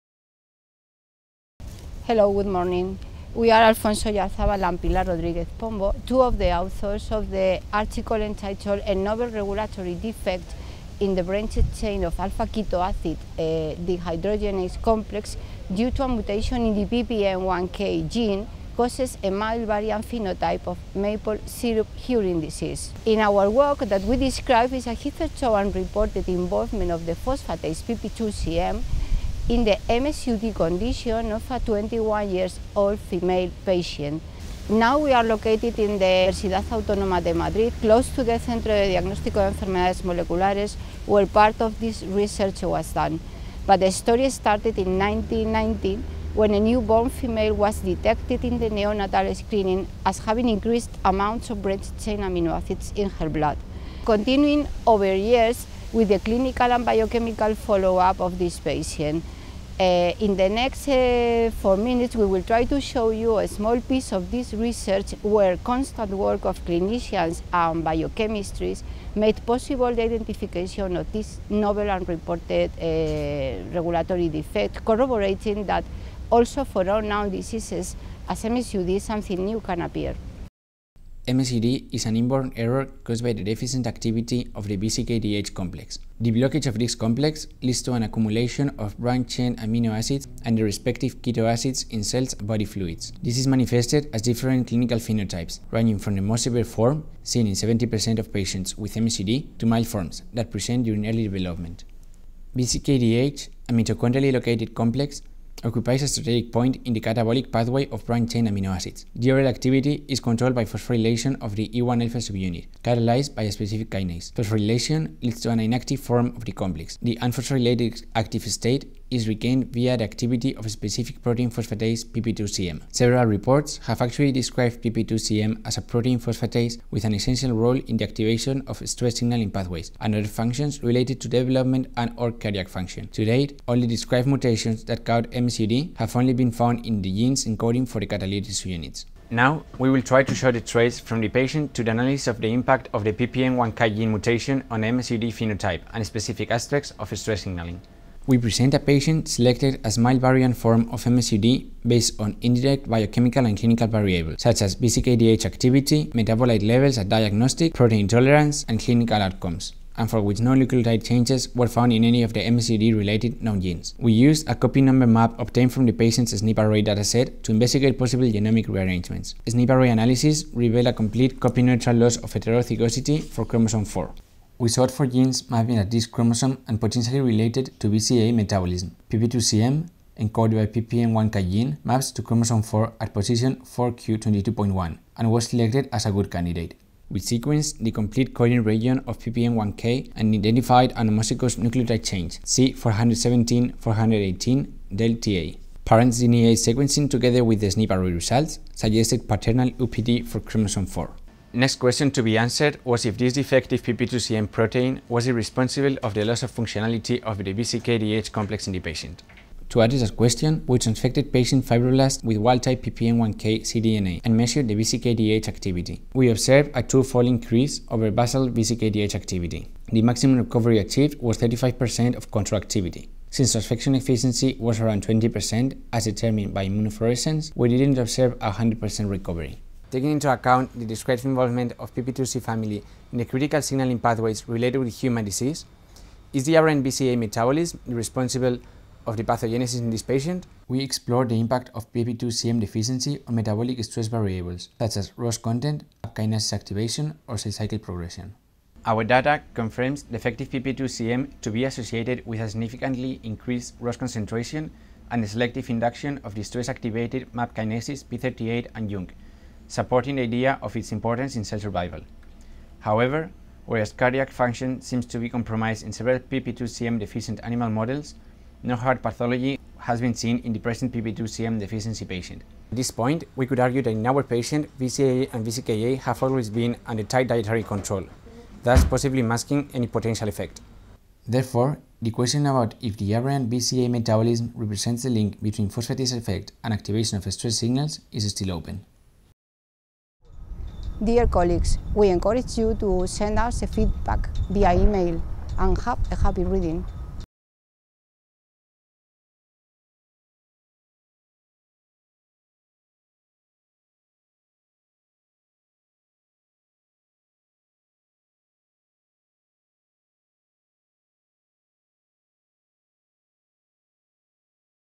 Human Mutation - Pilar Rodríguez-Pombo and Alfonso Oyarzabal
Video Highlight: Authors Pilar Rodríguez-Pombo and Alfonso Oyarzabal on their recently published Human Mutation paper entitled "A Novel Regulatory Defect in the Branched-Chain Alpha-Ketoacid Dehydrogenase Complex Due to a Mutation in the PPM1K Gene Causes a Mild Variant Phenotype of Maple Syrup Urine Disease".